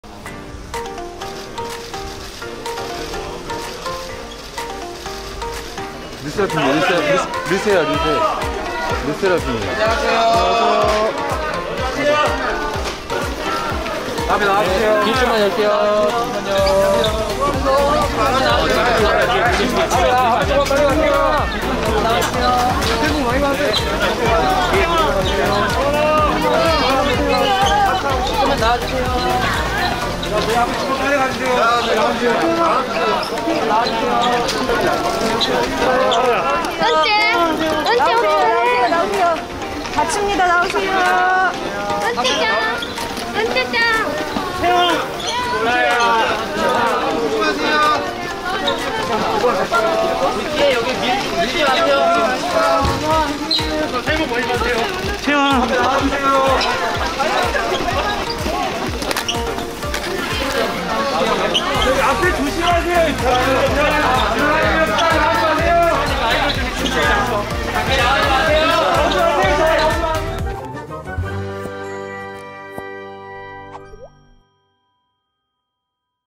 르세라핌(LE SSERAFIM) 김포공항 입국 | LE SSERAFIM Airport Arrival
Group LE SSERAFIM arrived from Japan through Gimpo International Airport on the afternoon of January 1 after completing their overseas schedule.